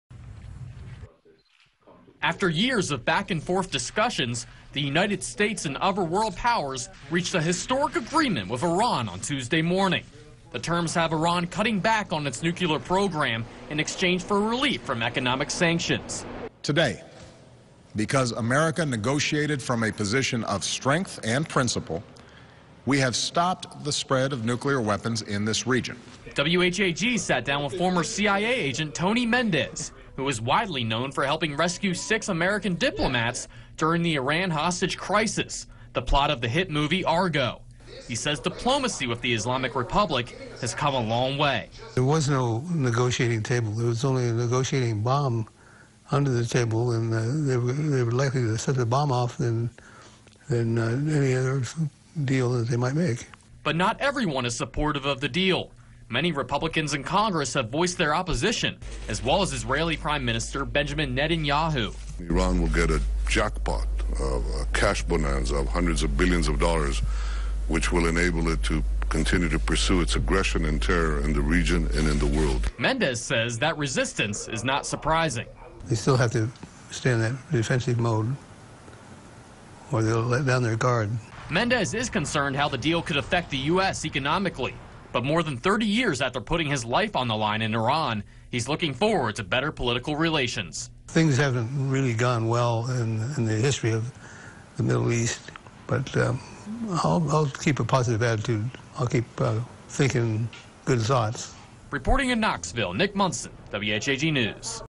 "Argo" Hero Reacts to Iran Deal
Former CIA agent, "Argo" hero Tony Mendez reacts to Iran nuclear deal.